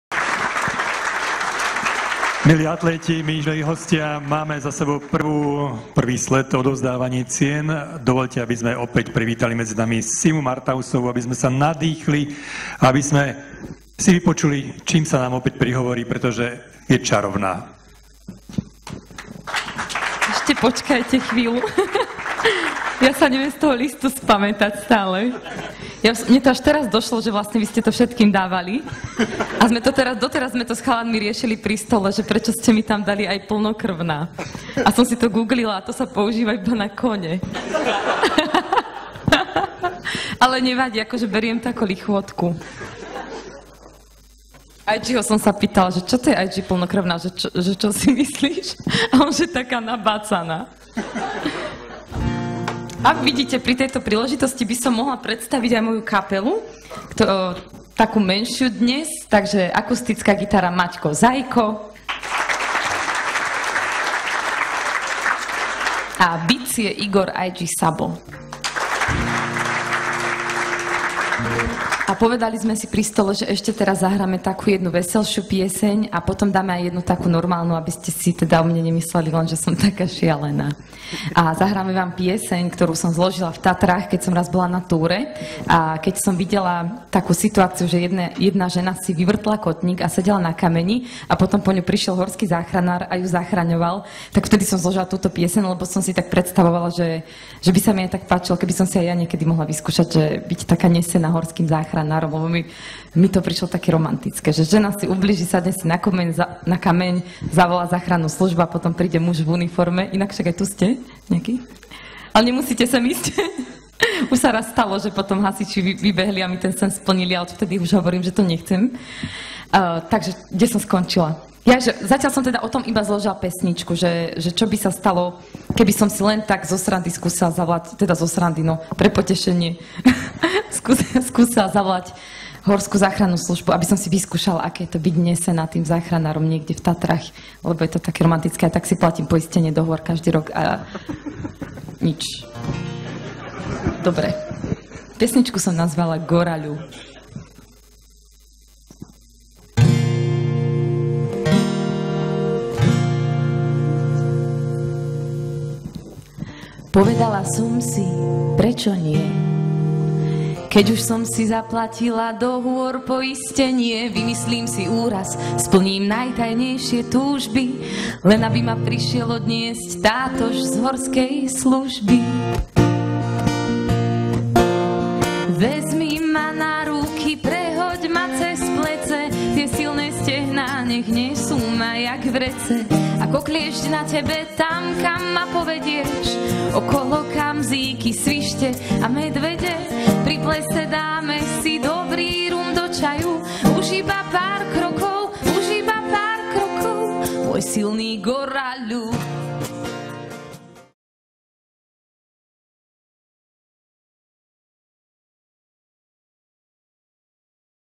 Atlét roka 2018 16 časť Minútka pre S Martausovú
Dnes náš seriál trochu odľahčíme a necháme priestor aj pre známu slovenskú speváčku, ktorá mala v priebehu večera viacero vystúpení - Simonu Martausovú. Tento šot nie je o jej pesničke, ale jej milom prejave.